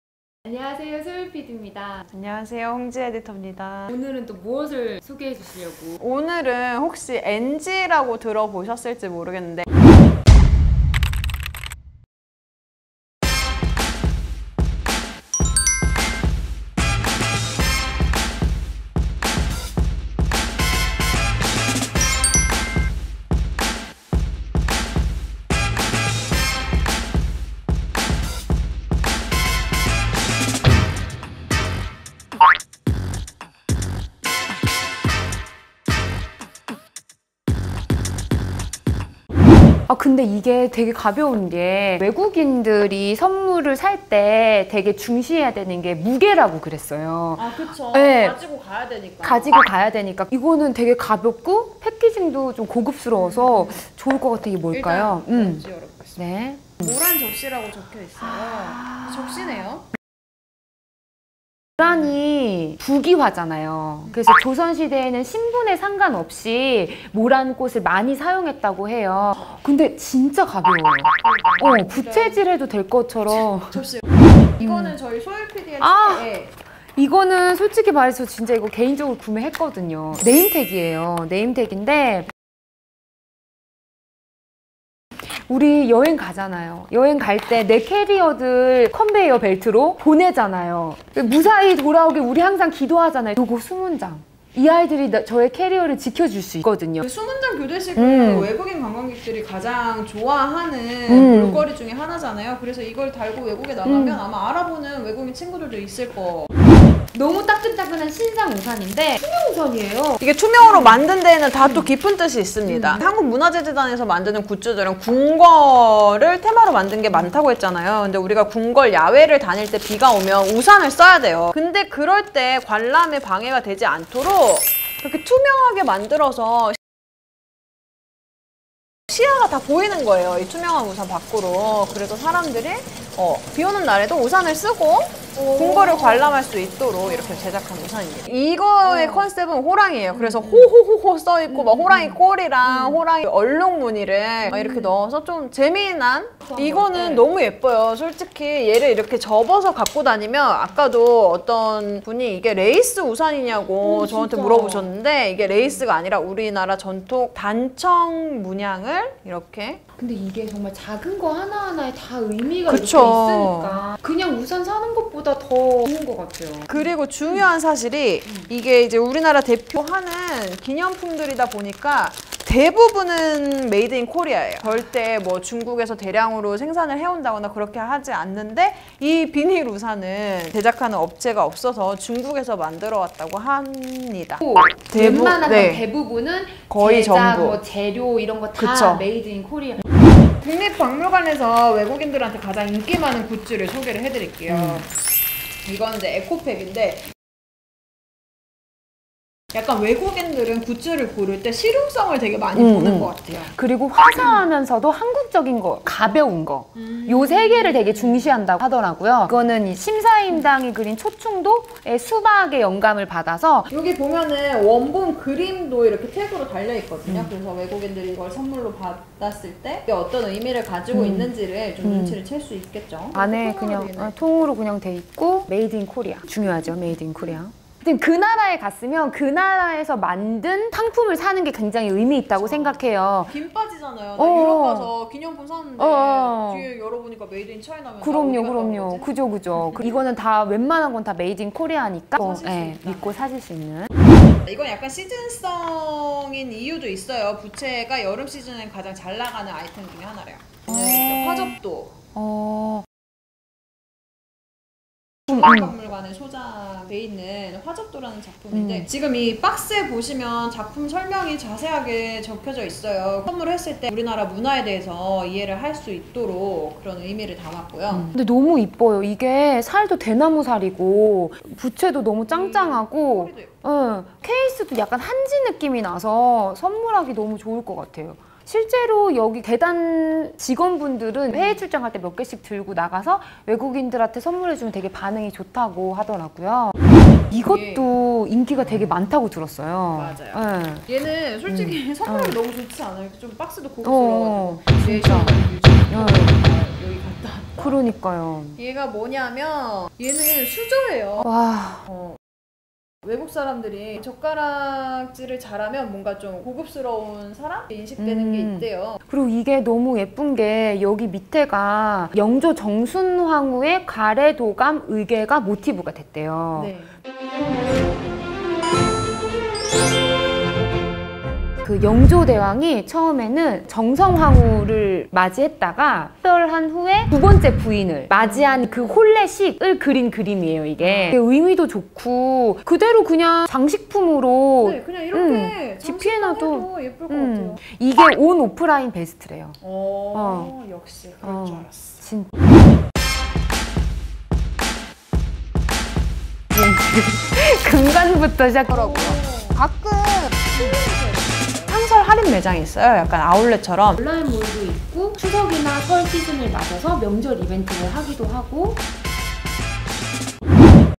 외국인 선물 고르는 방법! 선물 추천 리스트✨ 박물관 굿즈 내셔널 굿즈 갓템🎁
나라가 만든 기념품이 이렇게 예쁘기 있긔없긔?😝
수문장 캐릭터 네임택, 부귀화인 모란이 새겨진 접시세트, 힙-한 우산 BAMMMM🌋
의궤수저세트, 신사임당 초충도가 현대적으로 그려진 에코백, 박물관이 소장하고 있는 작품이 새겨진 전통부채까지!!
🎁외국인 친구에게 선물하기 좋은 내셔널 굿즈 리뷰
🌈외국인 선물 제대로 고르는 꿀팁도 있으니 시청필수🌈

기획, 출연 = 홍지, 소율
촬영 = 소율, 정욱
편집 = 소율
이미지 출처 = 게티이미지뱅크

취재협조 = 국립박물관문화재단, 한국문화재재단
촬영장소 = 한국의 집 청우정

앞으로 업로드될 다양한 여행 영상을 보고 싶다면 구독! 좋아요! 많이많이 눌러주세요!❤️